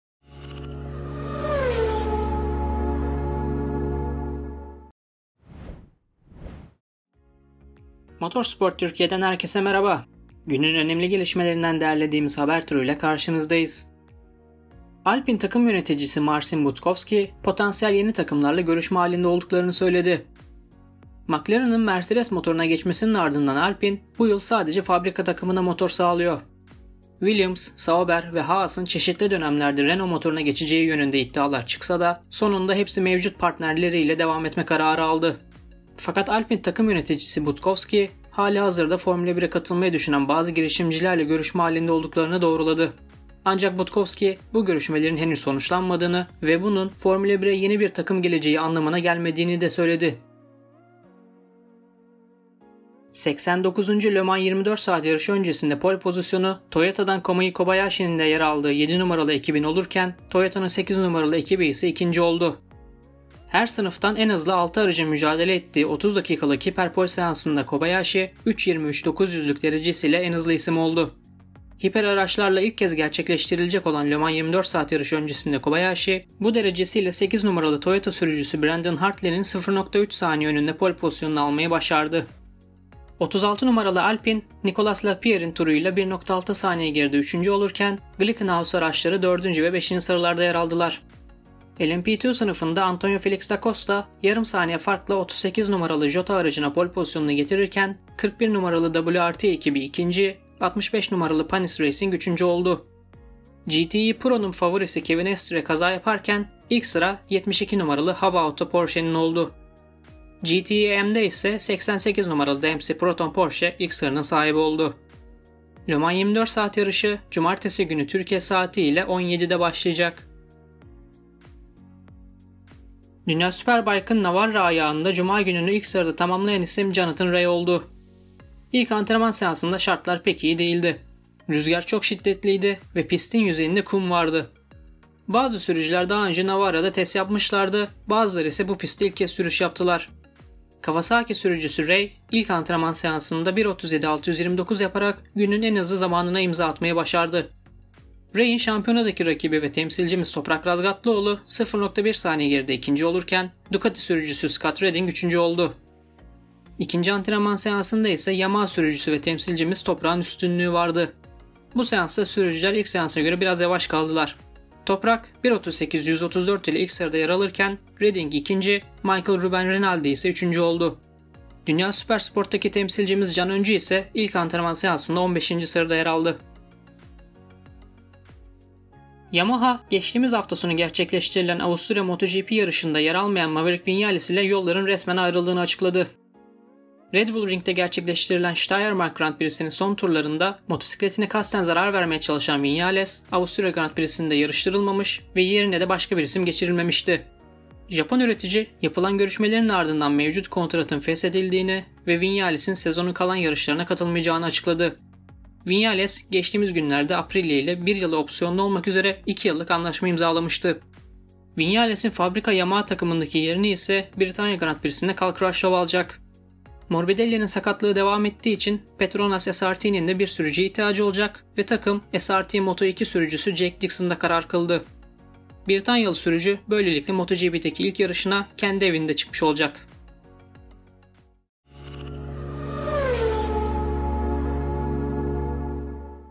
Alpine potansiyel yeni takımlarla görüşüyor! - 20 Ağustos 2021 Günün Önemli Motor Sporları Haberleri
Alpine takım yöneticisi Marcin Budkowski, potansiyel yeni takımlarla görüşme halinde olduklarını söyledi.

Bu ve bunun gibi daha fazla içeriğe ulaşmak için sitemizi ziyaret edebilirsiniz